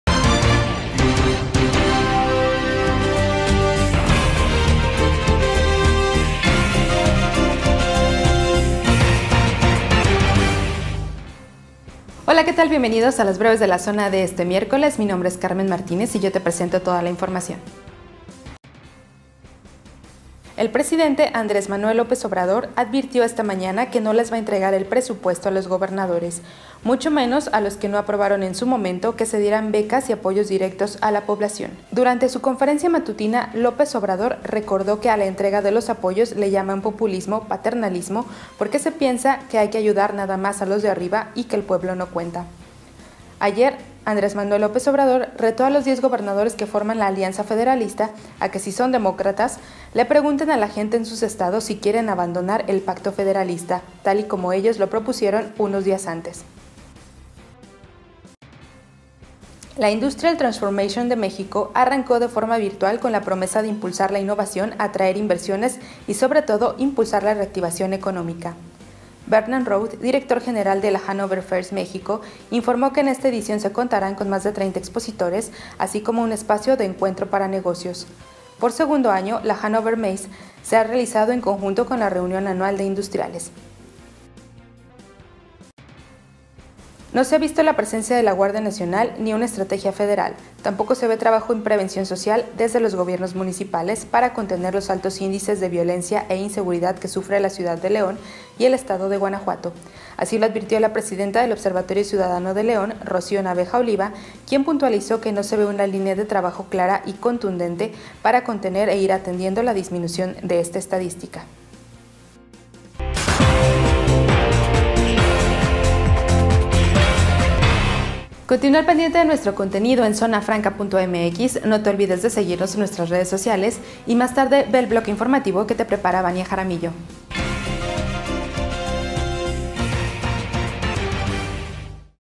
AMLO revira a gobernadores: ¿Qué no votaron contra pensiones y becas? | Breves de la Zona
- Ausencia federal y nula prevención del delito en lo local, mantienen crisis de inseguridad.
- En formato virtual ITM promete empujar reactivación económica.
- AMLO revira a gobernadores: ¿Qué no votaron contra pensiones y becas? No les daremos ese dinero.